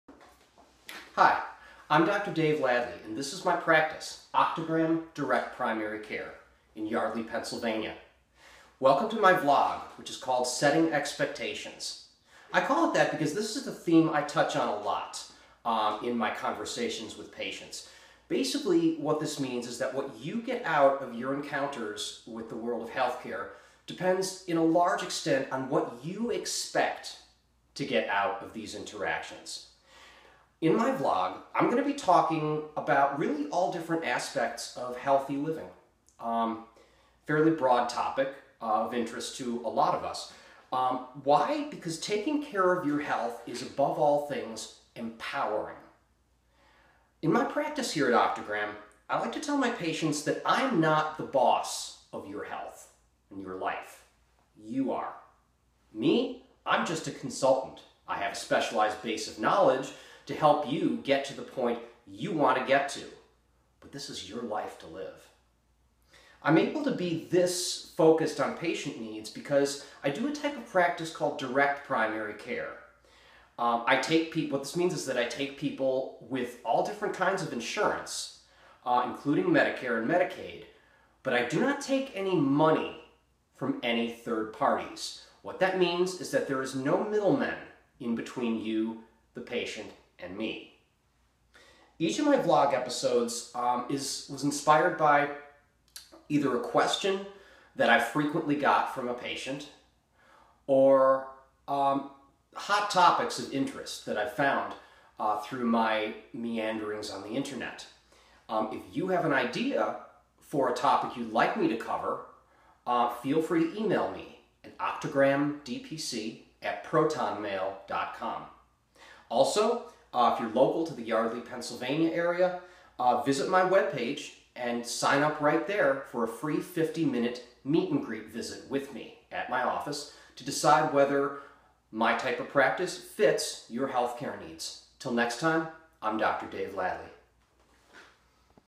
Setting Expectations -- Introduction
Welcome to my vlog, where I'll be talking about empowering yourself through healthy living. to schedule a free 50min meet and greet visit at my office in Yardley, Pennsylvania.